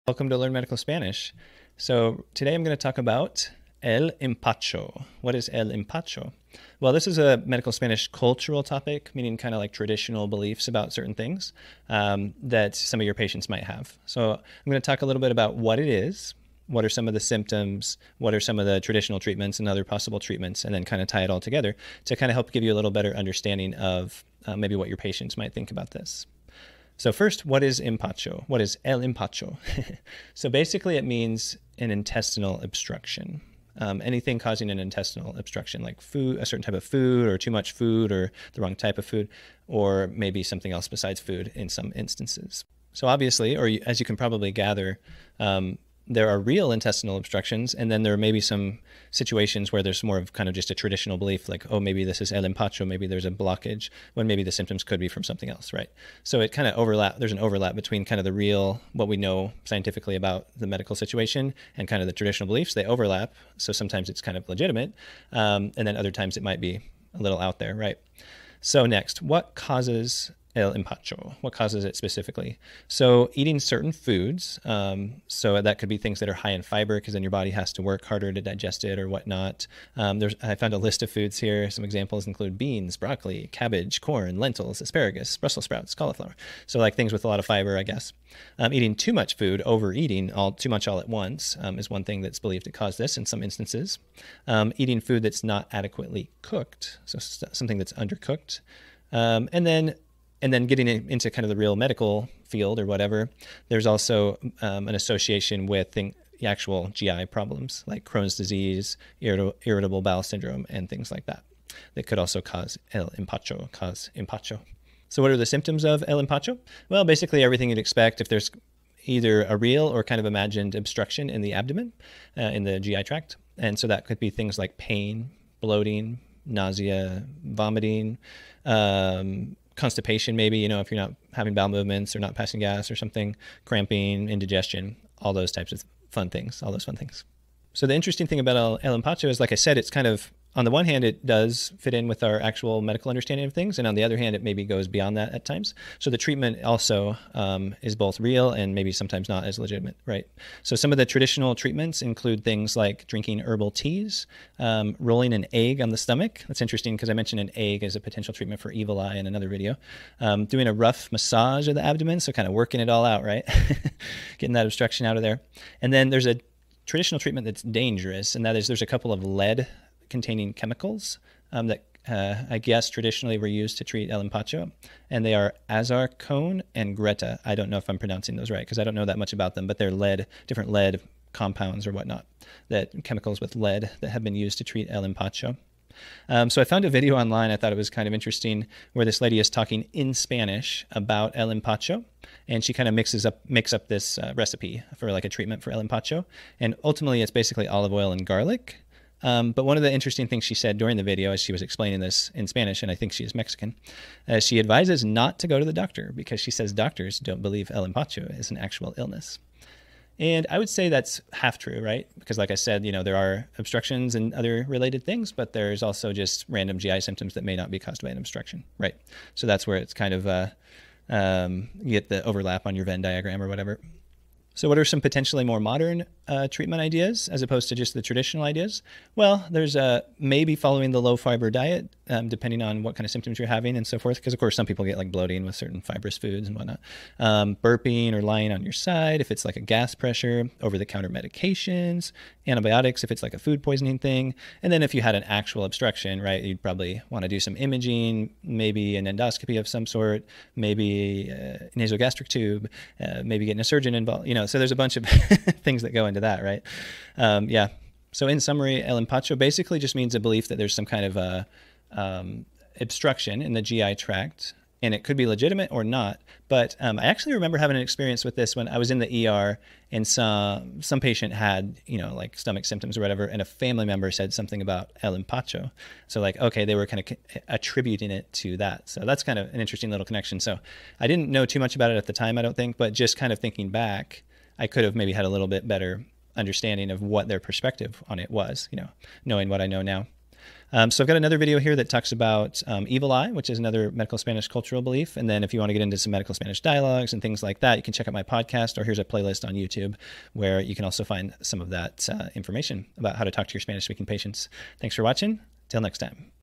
What is "El Empacho"? [Medical Spanish Cultural Topic]
"El empacho" is a Spanish phrase that loosely translates to indigestion, or impaction of the GI tract. GI symptoms are often attributed to el empacho, whether valid or not. Today I'll explain the causes, symptoms, and treatments of el empacho so you can better relate to your Spanish-speaking patients.

🎧 The “Learn Medical Spanish” podcast-

"El empacho" is the medical Spanish cultural belief regarding gastrointestinal obstructions, which may or may not be based in reality (depending on the situation). In today's video I explain what is believed to cause el empacho, as well as the symptoms, treatment (including some that are beneficial, and others that could be harmful), and more for el empacho. This info should be helpful when working with your Spanish-speaking patients from various parts of Latin America.